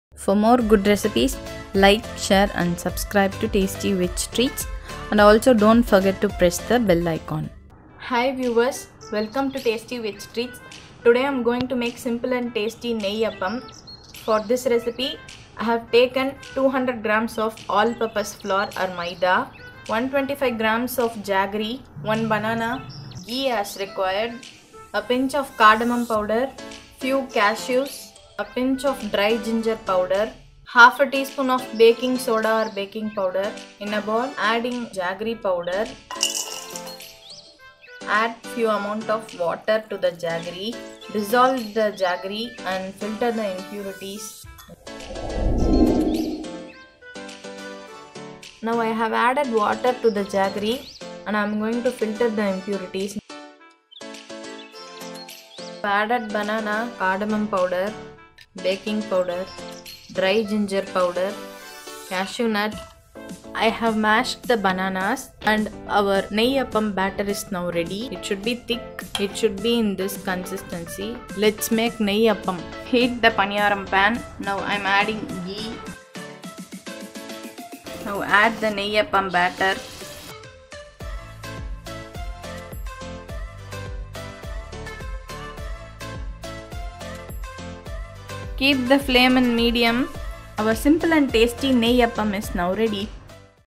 Simple and Tasty Nei Appam Recipe|Sweet Recipe|See the description for ingredients|Tasty Veg Treatz
Maida - 200gms
Jaggery - 125gm
Banana - 1
Cardamom powder -  pinch
Dry ginger powder - 1/4tsp
Cashews - few
Baking soda/powder - 1/2tsp

Method:

_ Add water to the jaggery,dissolve it and filter the impurities.
_ Then add it to the maida or all purpose flour.
_ Mash the banana and add all the remaining      ingredients except ghee.
_ Give the batter a nice mix.
_ Heat the paniyaram pan and add ghee.
_Then pour one scoop of  batter to each pit of the pan.
_Cook it for 2 to 5 minutes in medium flame.
_ Our simple and tasty nei appam is ready.